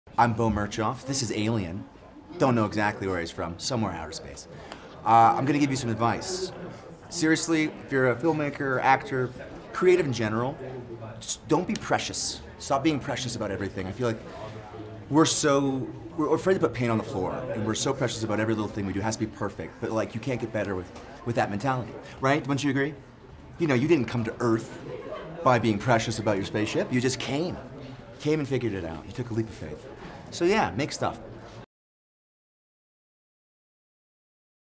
Beau Mirchoff | CreateYourStory at Sundance 2019 | Adobe
Beau Mirchoff stopped by the Indiewire Studio at Sundance Film Festival 2019 to share advice and inspiration on how young filmmakers can create their own stories.